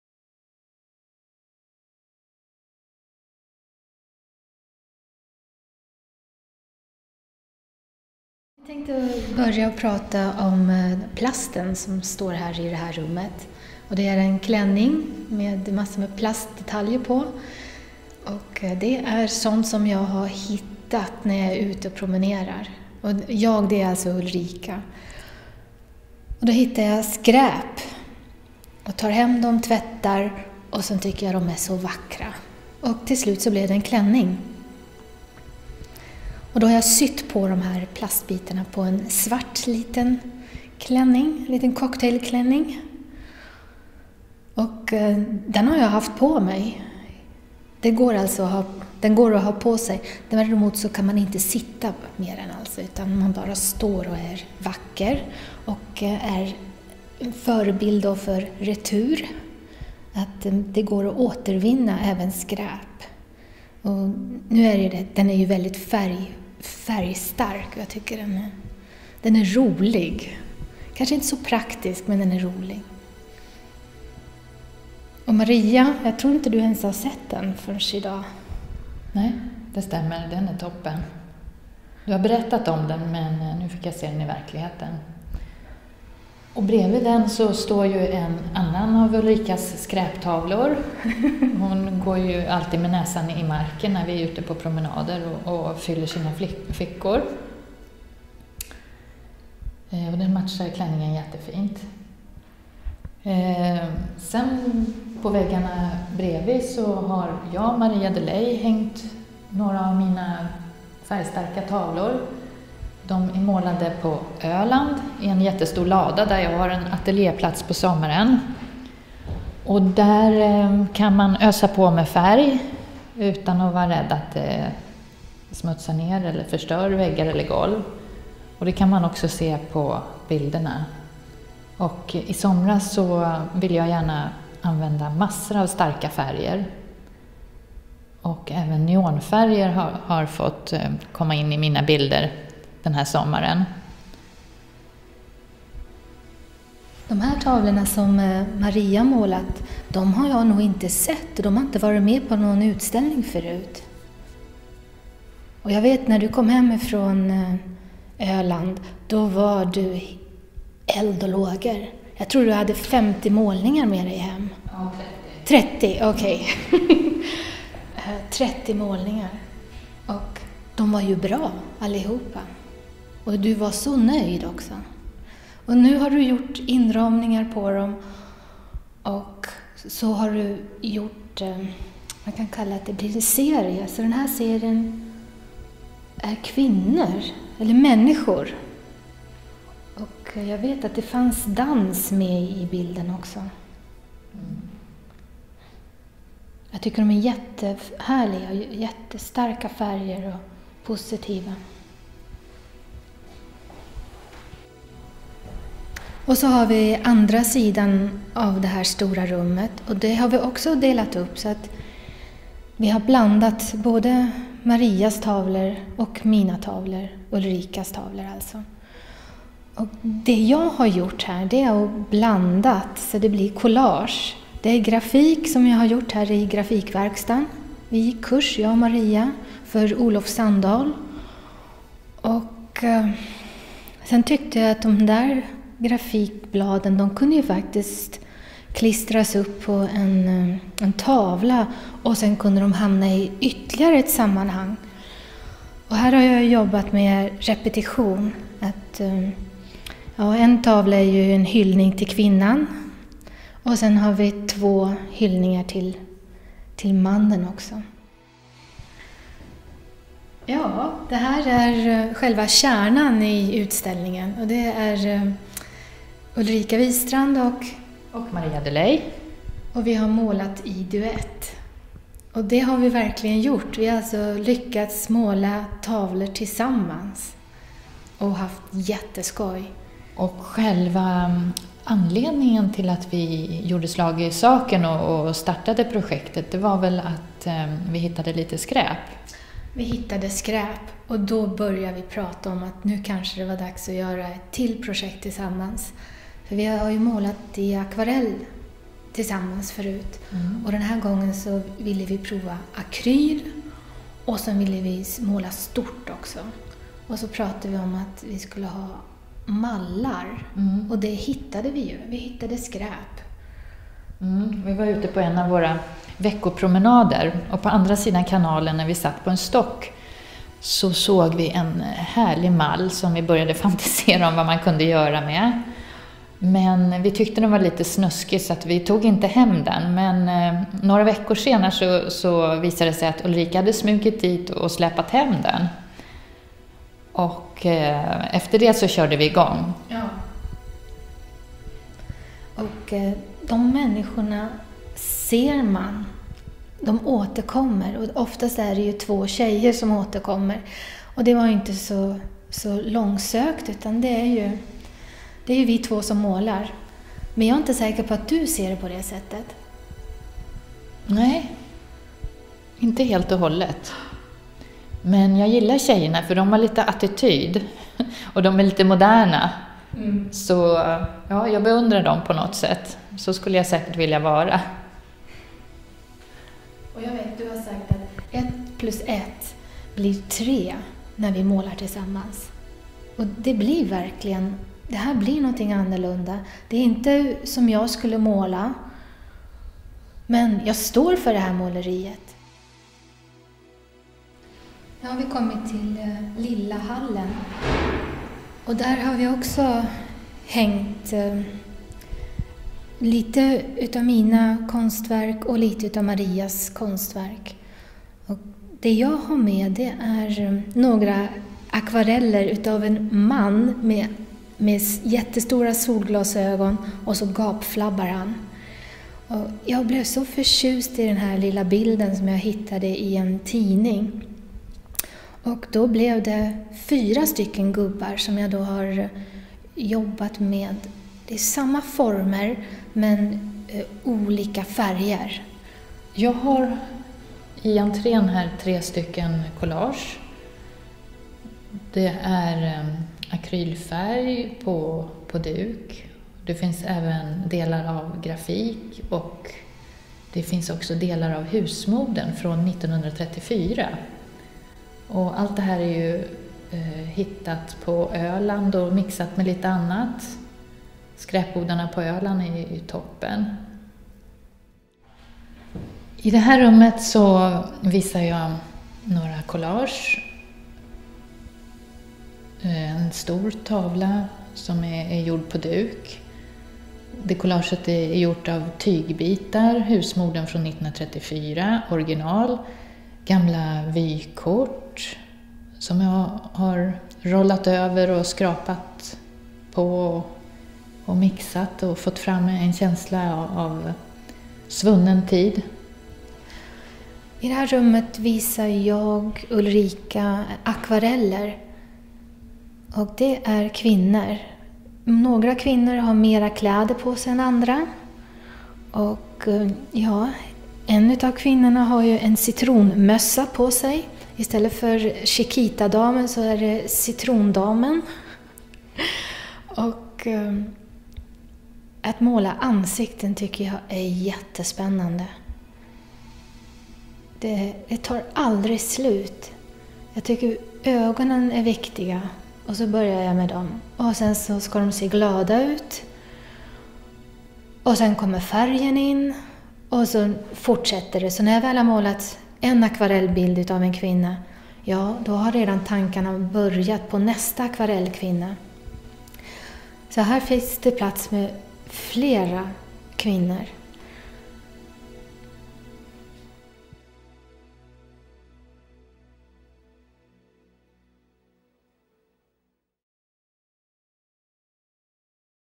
ETTPLUSETTBLIRTRE.mov
ETT PLUS ETT ÄR TRE

Ulrika Wistrands och Maria de Leye
13 februari kl 12 - 16

Konsthotellet, öppnar den 13 februari utställningen Ett plus ett är tre
Denna utställning är en del i en serie audioguidevisningar som Konsthotellet
erbjuder i samverkan med Galleri Kretsens utställningsprogram i Södertälje Konsthotellet är mycket glada att i samverkan med Galleri Kretsen,i Södertälje, presentera utställningenEtt plus ett är tre i Ulrika Wistrands och Maria de Leyes utställningMåla i duett.

Maria: Jag tycker Ulrika har gjort jättespännande saker, det är en mix av
många tidigare idéer som hon har haft, väldigt spännande mönsterkombinationer
Ulrika: Jag tänkte på de här mörka bilderna du har gjort Maria, det är ju
vågat att gå in i ett mörker. Oftast är det ju mera glada färger som finns i
måleriet. Men här vågar du gå in i mörkret.